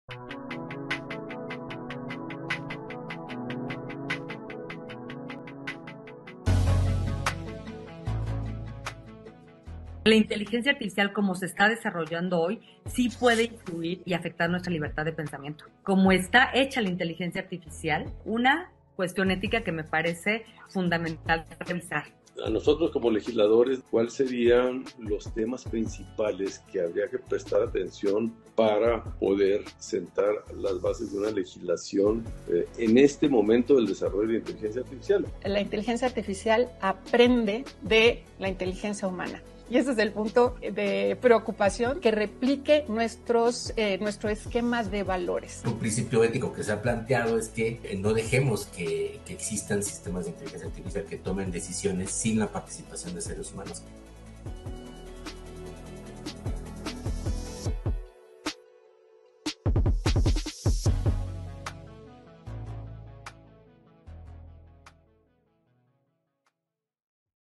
La Inteligencia Artificial en México: un desafío ético y tecnológico / Senado de la República
En la Cámara de Senadores se debate el futuro de la inteligencia artificial a través de foros como en el que recientemente participaron la senadora @alelagunes y el senador @GustavoMadero junto a académicos e investigadores.